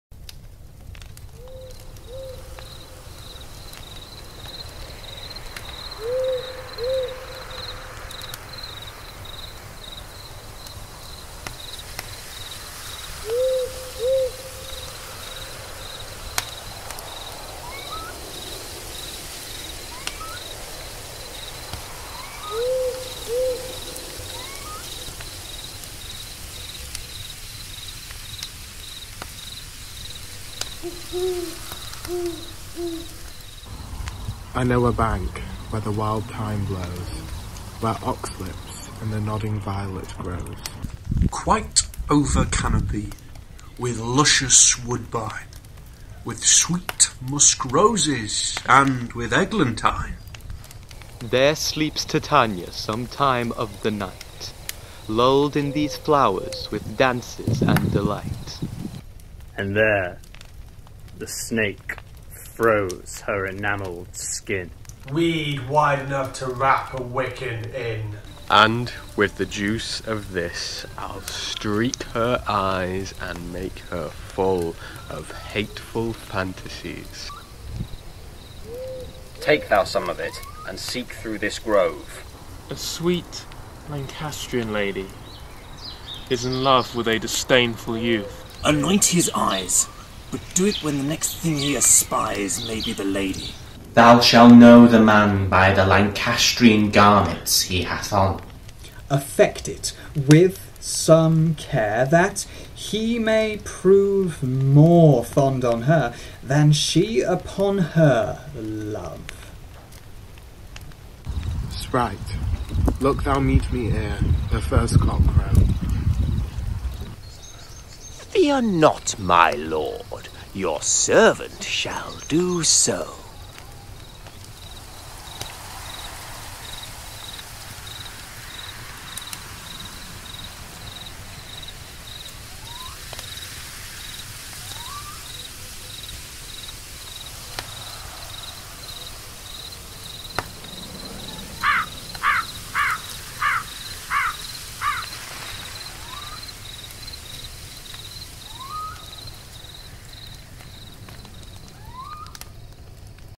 A Midsummer Night's Dream - Oberon's Monologue - Performed by Three Left Feet
To introduce your Sunday, we have some amazing members from Three Left Feet Theatre Company performing "All The Worlds A Stage from the the Shakespeare play As You Like It.
We hope this brightens your evening!

In a time of uncertainty, our aim of the project is to raise money for The Globe Theatre London and other local theatres in the North West who have had to close their doors on audiences due to the COVID- 19 epidemic.

Here is a link to our crowdfunder page if you would like to donate to support our amazing arts organisations!